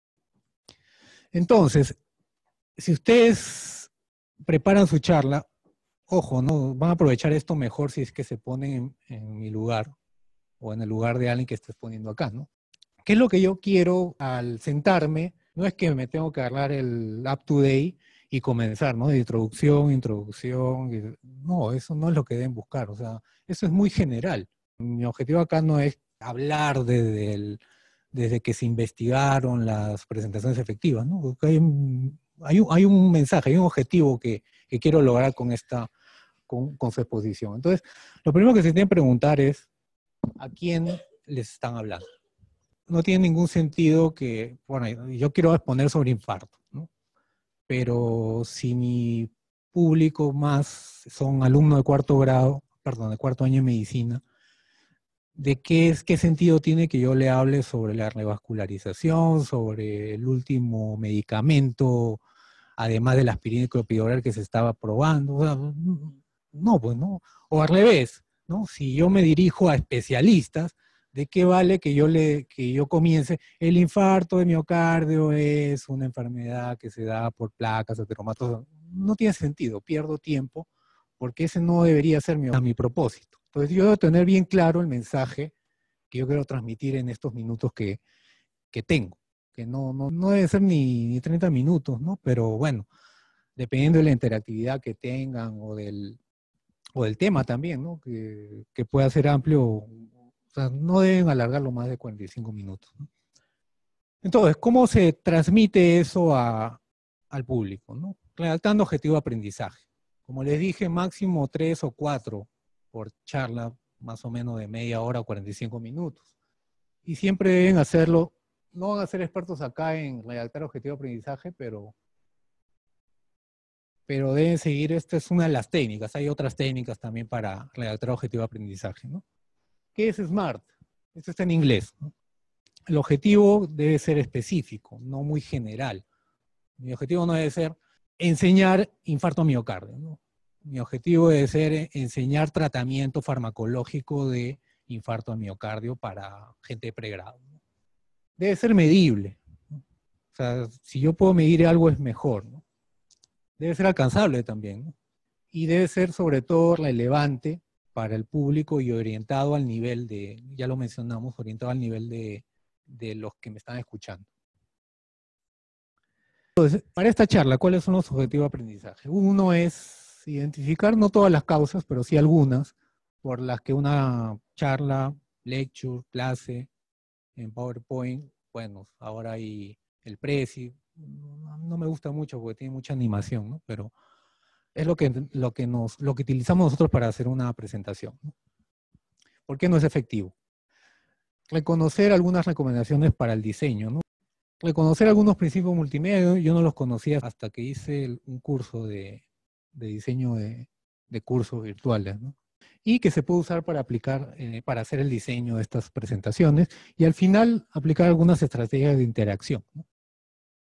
Objetivos de aprendizaje Cómo redactarlos 13 noviembre 2018
En base a la técnica SMART explicamos cómo se deben redactar los objetivos de aprendizaje que debo tener para diseñar una charla o presentación.